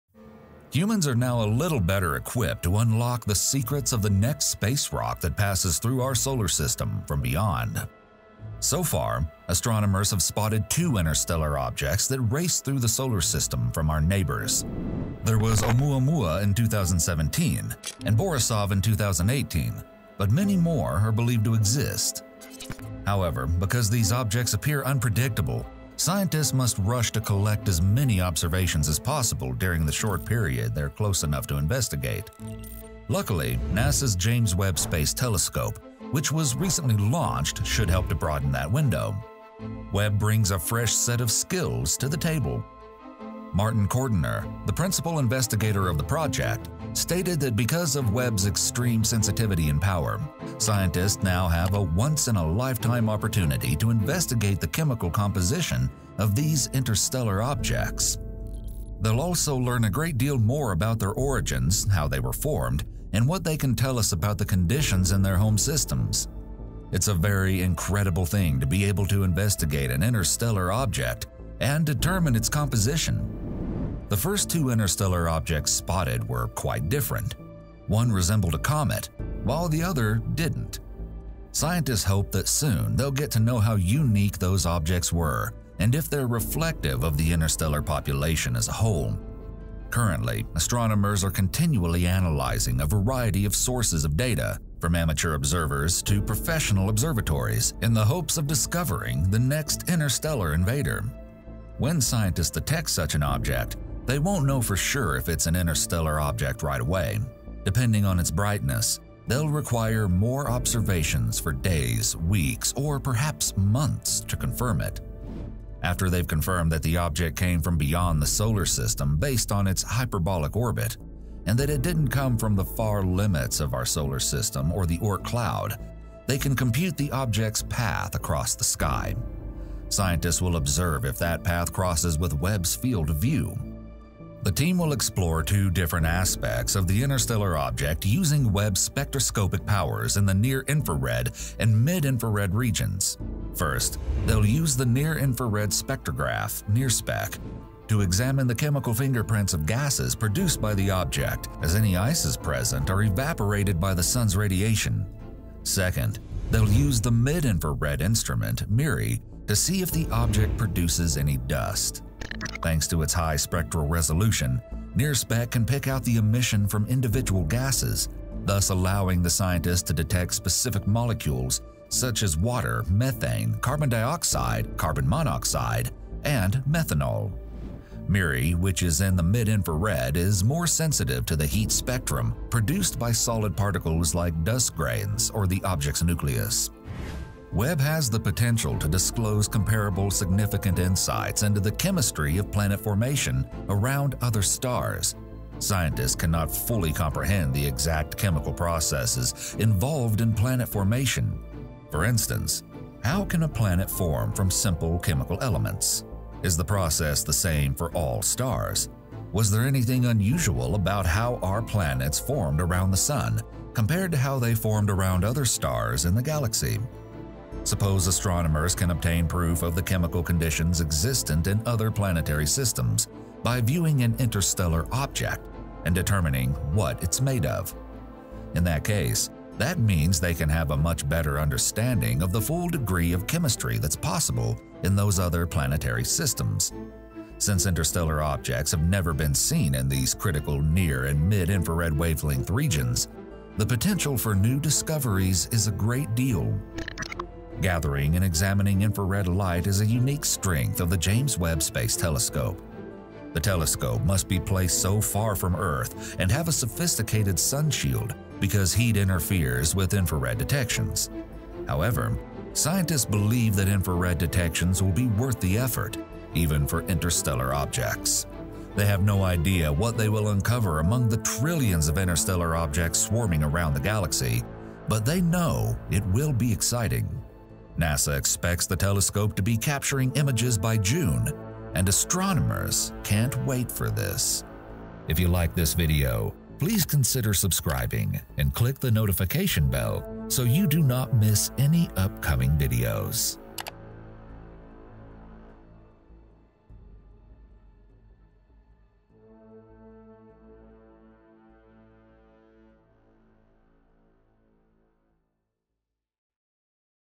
Studying The Next Interstellar Object With Webb Telescope
NASA’s James Webb Space Telescope will help unlock the secrets of the next space rock that passes through our solar system from beyond.

Produced & Edited by:
Ardit Bicaj

Written by:
Nicole Amondi

Narrated by:
Russell Archey

Space Engine
NASA's Goddard Space Flight Center
NCSA, NASA, A. Boley

A big thank you to our lovely members:
Joseph Pacchetti